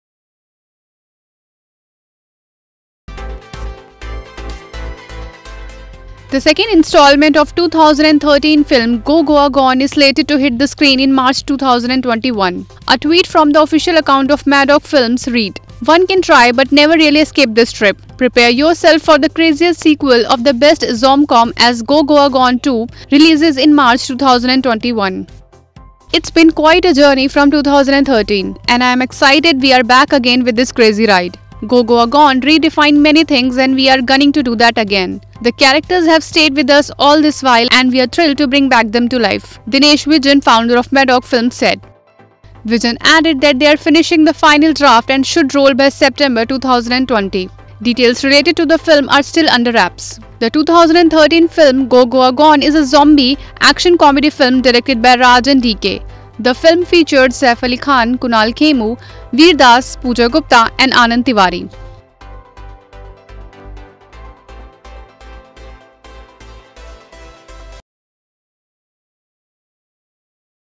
'Go Goa Gone 2' to release in March 2021
The second installment of the 2013 film "Go Goa Gone" is slated to hit the screen in March 2021.